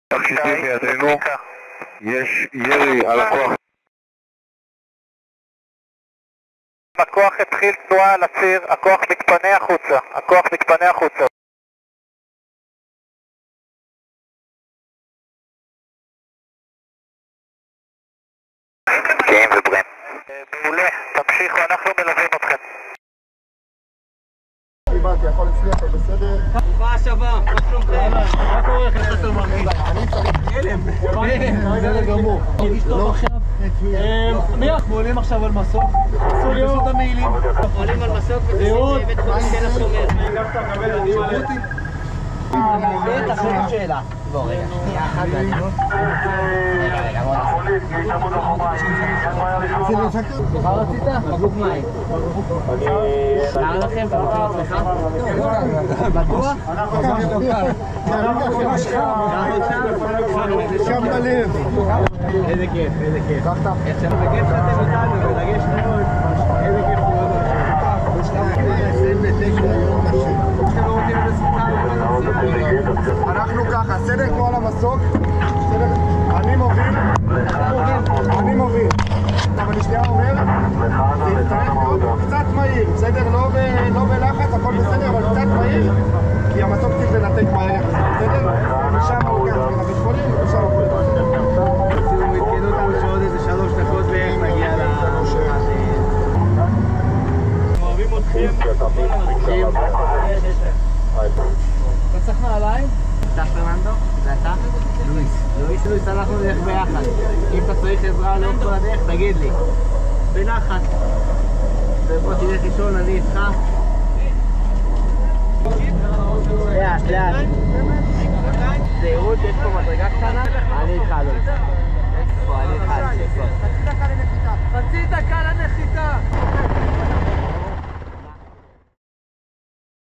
Moment Israeli special forces liberate two hostages during overnight Rafah rescue operation
The IDF has unveiled new video footage from the recent overnight hostage rescue operation in Rafah, southern Gaza. The footage captures special forces facing gunfire and escorting Fernando Simon Marman and Norberto Louis Har to a helipad in the Strip alongside the Navy's Shayetet 13 commando unit.

An officer can be heard on the radio announcing, "The hostages are in our hands, there is gunfire against the force." Another officer reports, "The force has begun movement on the axis; the force is heading out," while the video displays a convoy of armored personnel carriers driving in Gaza.